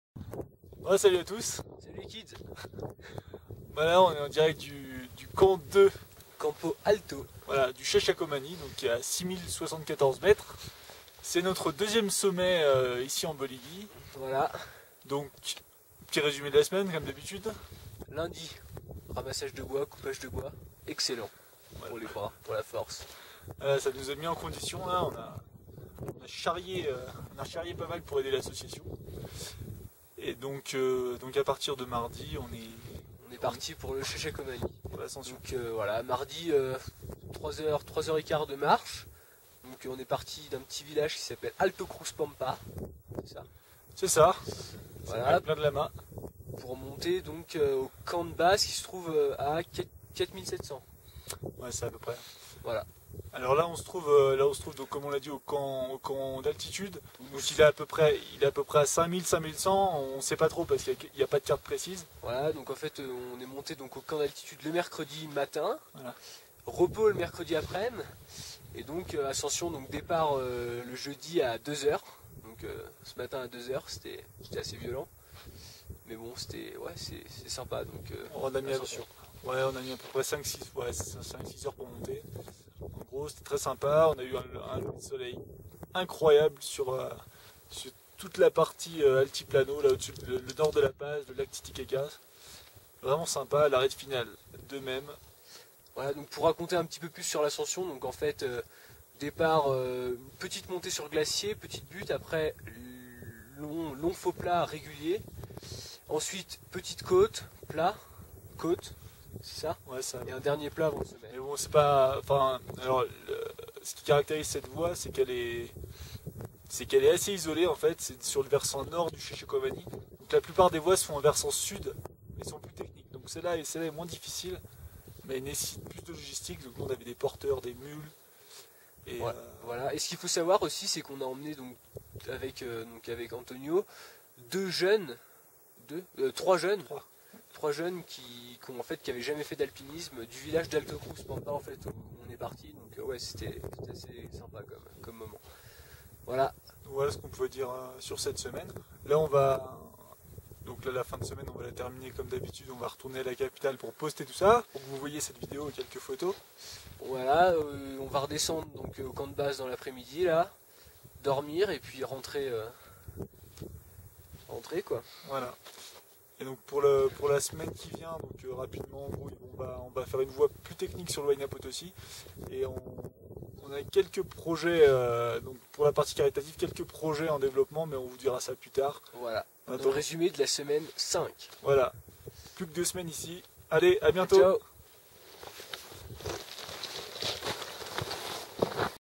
Expédition Bolivie 2012 - Résumé Semaine #4
Résumé de notre semaine 4 en Bolivie, du ramassage de bois à l'ascension du Chachacomani (6074m).

🤵 × Alex & Paul